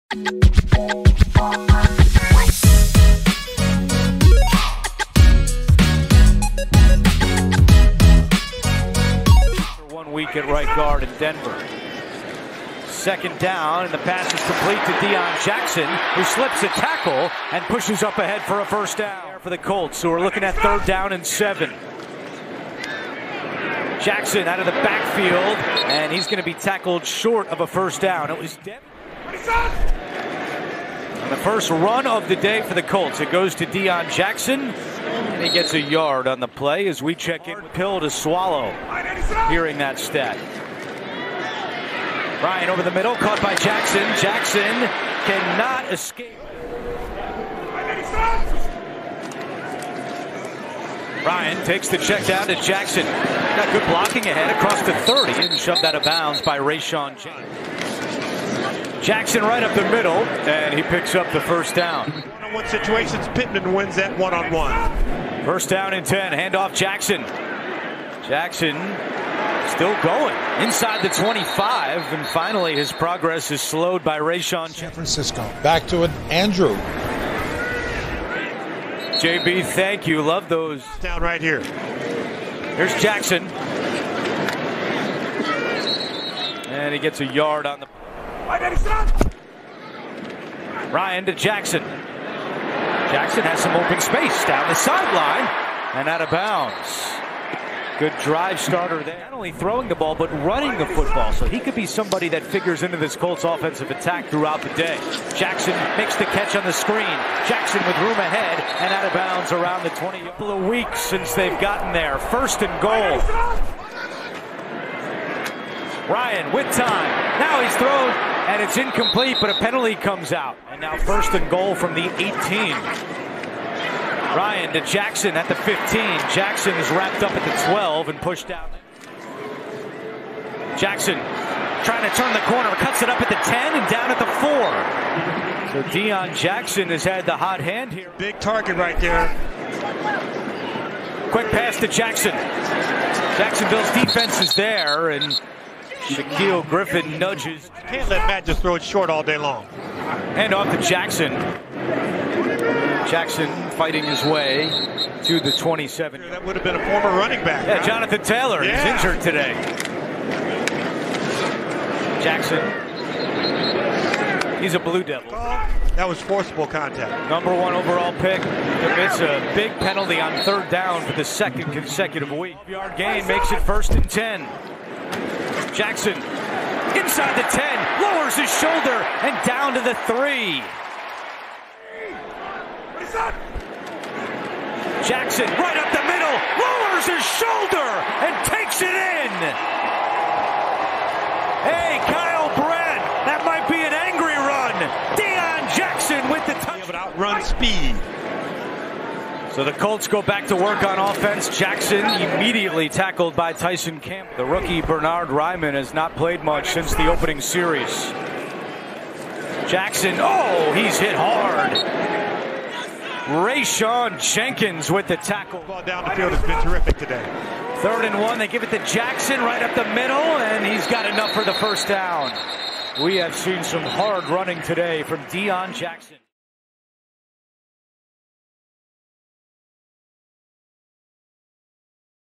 Deon Jackson RB Indianapolis Colts | Every play | 2022 | Week 6 vs Jacksonville Jaguars
Deon Jackson RB Indianapolis Colts

2022 Week 6 vs Jacksonville Jaguars

Rushing: 12 carries, 42 yards, 1 touchdown
Receiving: 10 targets, 10 receptions, 79 yards